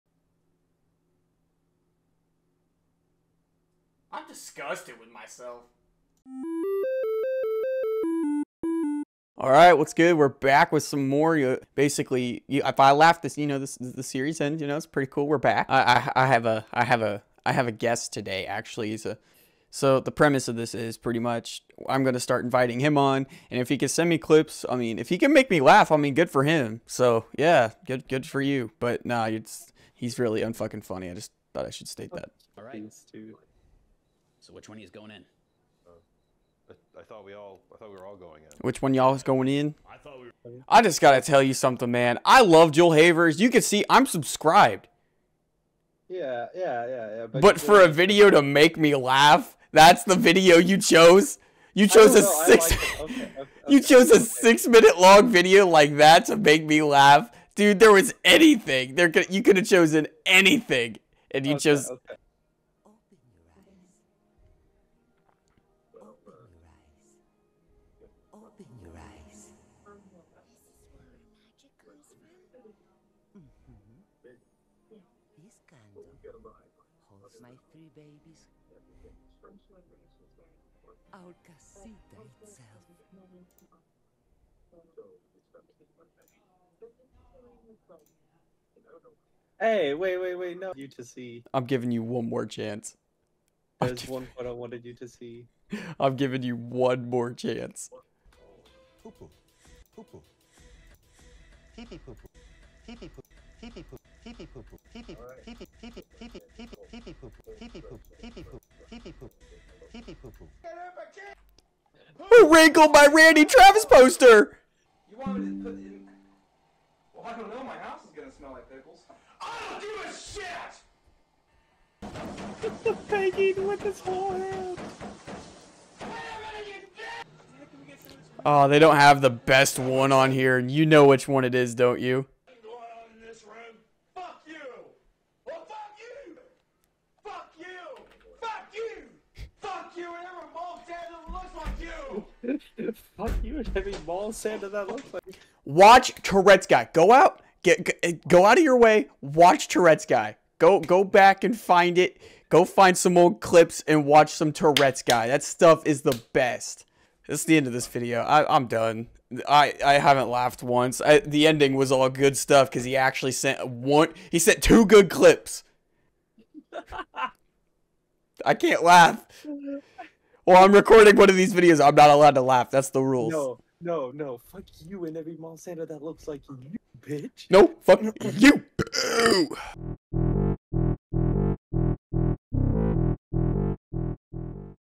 If I Laugh The Series Ends #3
In this weird video, I try not to laugh.
A laugh to me consists of a lough sound emitted from my mouth.